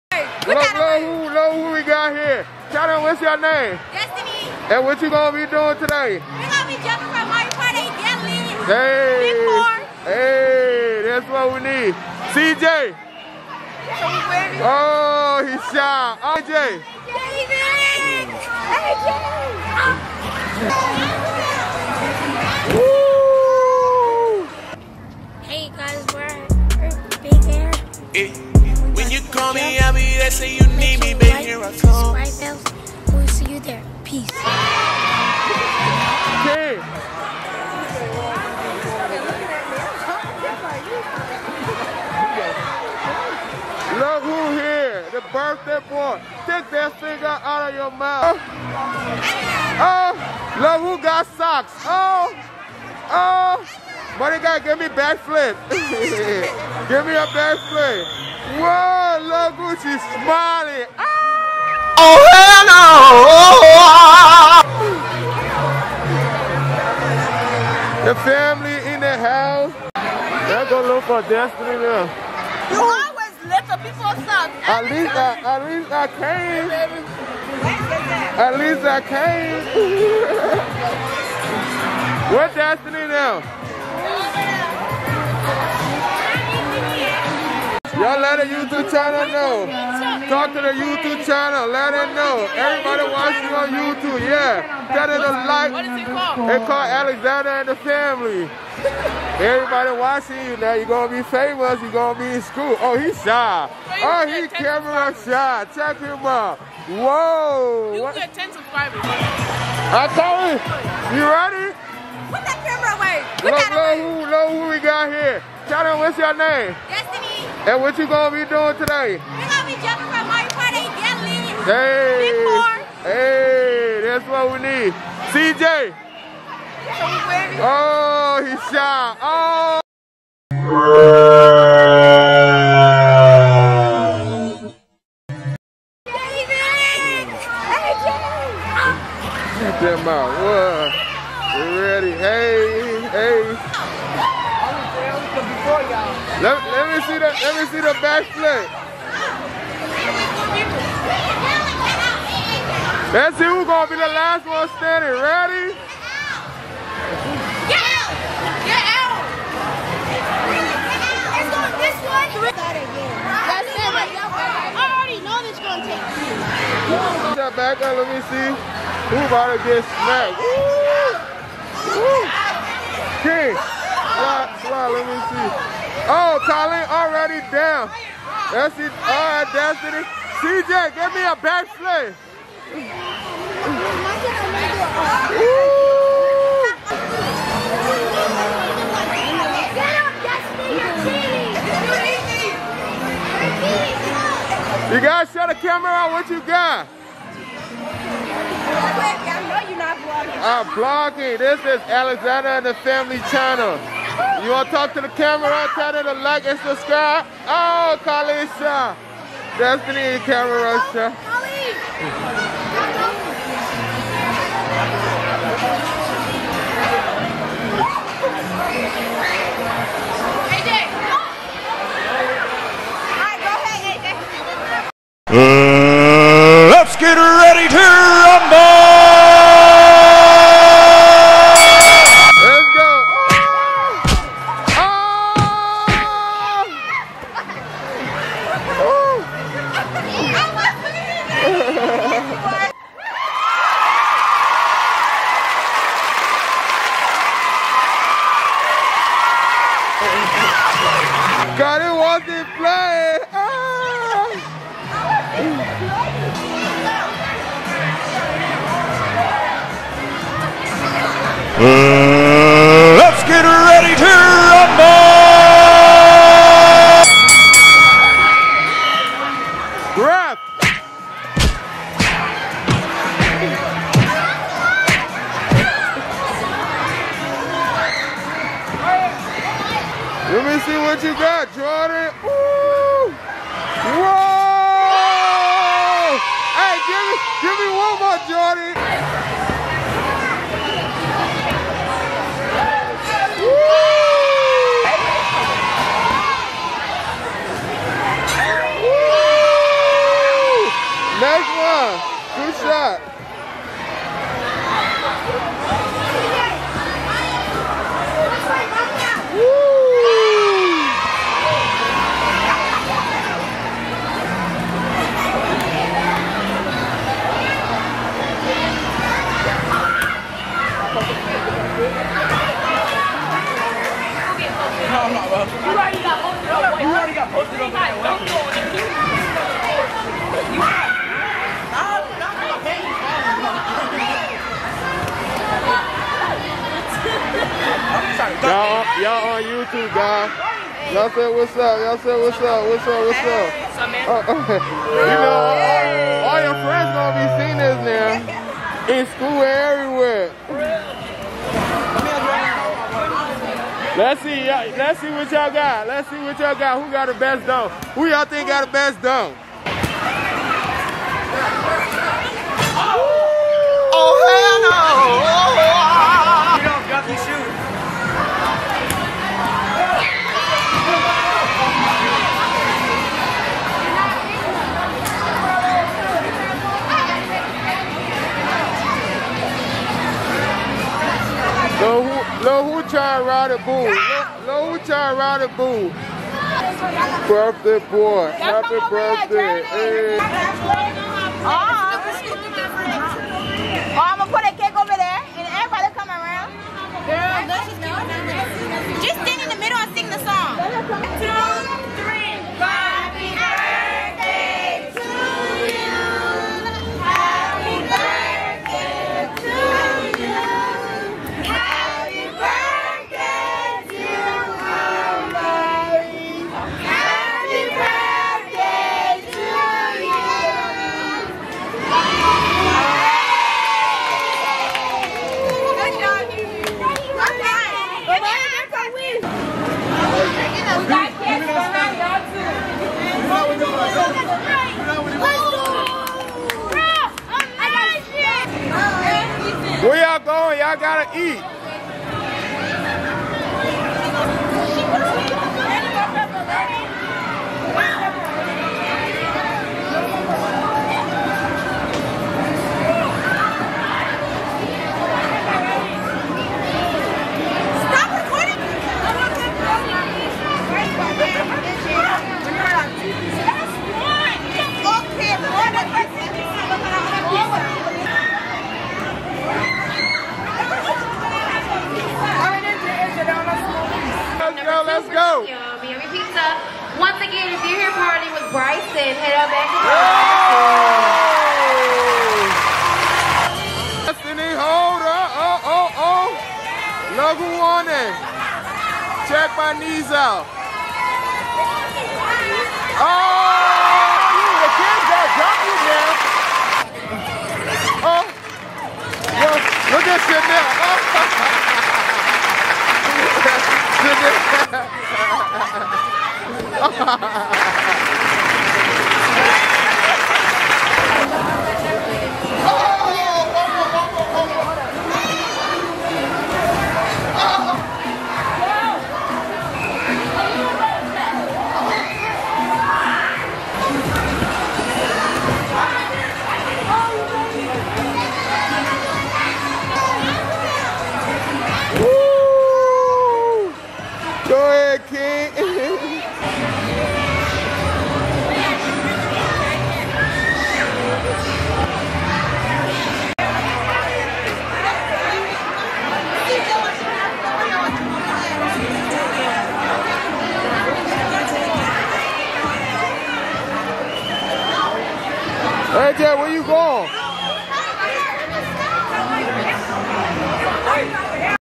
WHAT HAPPEN AT OMARI BIG 4 BIRTHDAY PARTY
ME AND THE FAMILY AT BIG AIR FOR OMARI BIG 4 BIRTHDAY PARTY